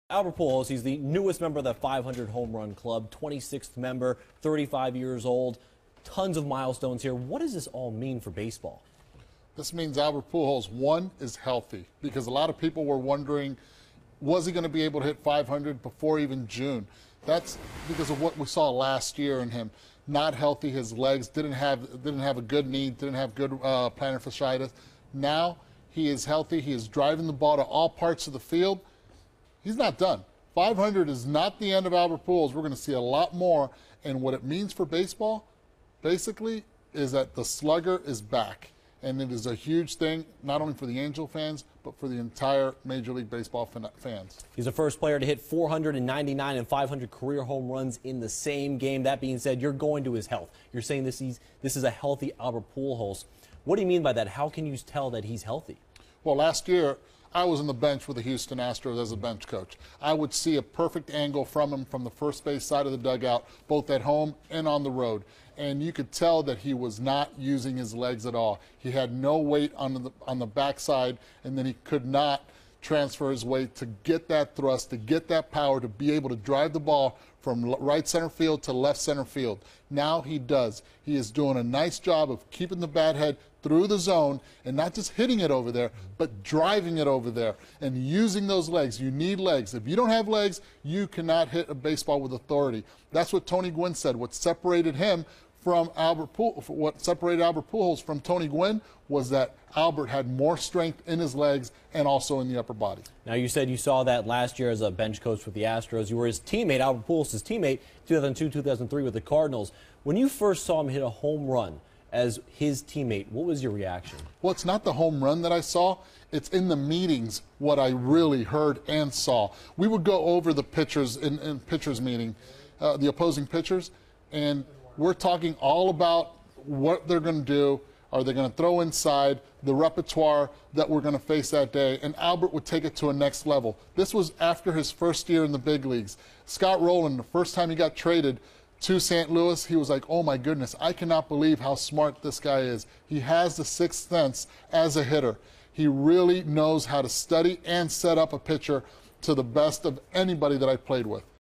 Pujols Hits 500th Home Run
Eduardo Perez breaks down what Albert Pujols hitting his 500th career home run means for him and Major League Baseball.